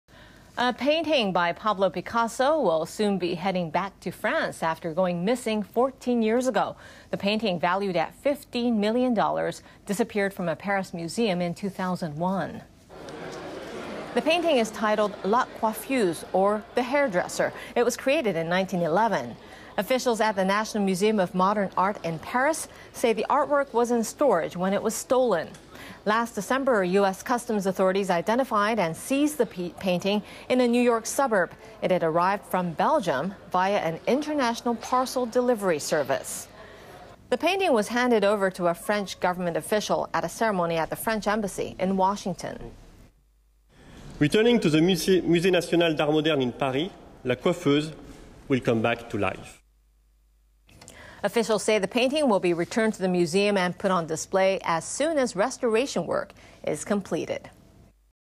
US returns stolen Picasso painting to France
The United States has returned to France a painting by Pablo Picasso that was stolen 14 years ago from a Paris museum.